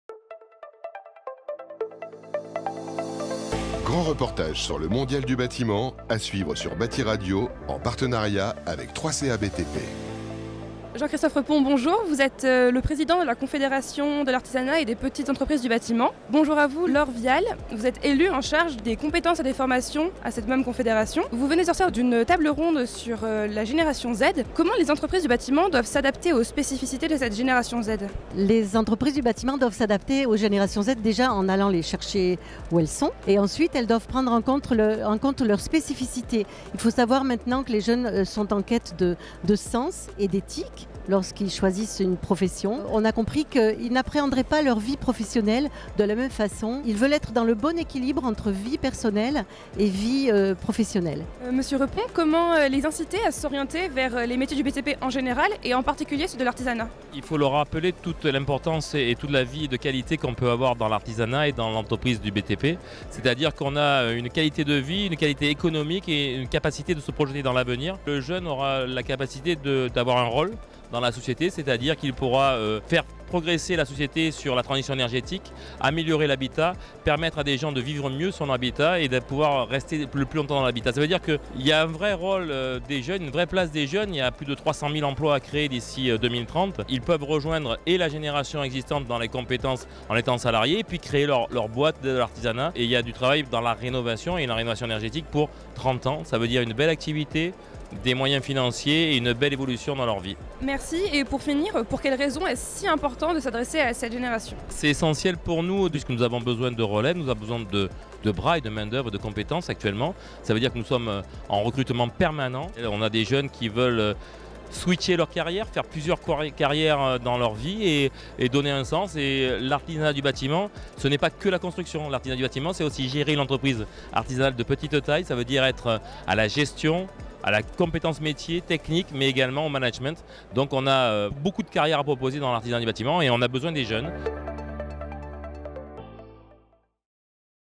CCCA-BTP sur BATIMAT - Attractivité des métiers et Génération Z
Avec Franck Le Nuellec, Directeur Marketing, développement et innovation stratégique, CCCA-BTP

Jean-Christophe Repon, Président de la confédération de l'Artisanat et des petites entreprises du bâtiment

Laure Vidal, élu en charge des compétences et des formations